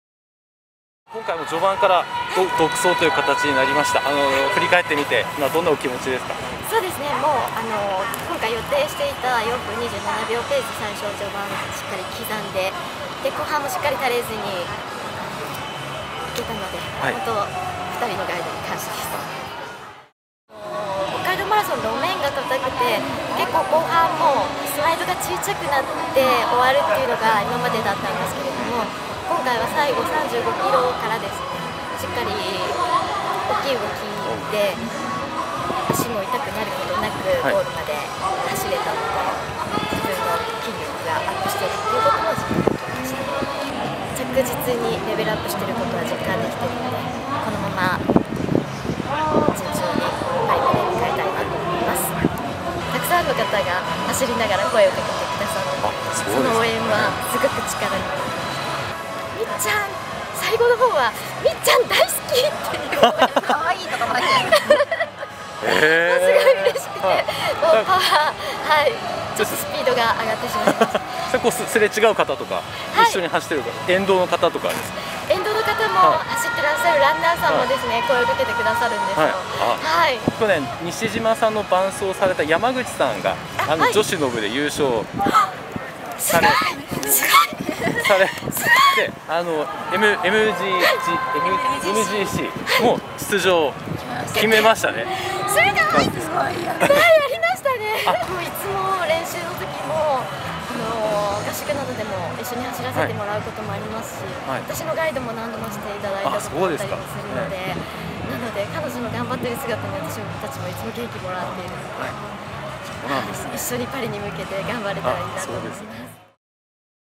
視覚障がい者の部女子で３連覇
金メダリストの力を、道下が見せつけた。視覚障がい者の部女子で２位に２０分以上の差をつけ「予定のペースをしっかり刻み、後半も落とさずに走れた」。東京パラリンピック前のスピード強化で、走りはさらに力強くなった。「北海道マラソンは路面が硬くて（疲労がたまり）、今までは後半にストライドが小さくなっていた。きょうは最後までしっかりと大きな動きができた」。４５歳の今なお、成長を続けている。「このまま（2024年パラの）パリに向かいたい」（2022/08/28）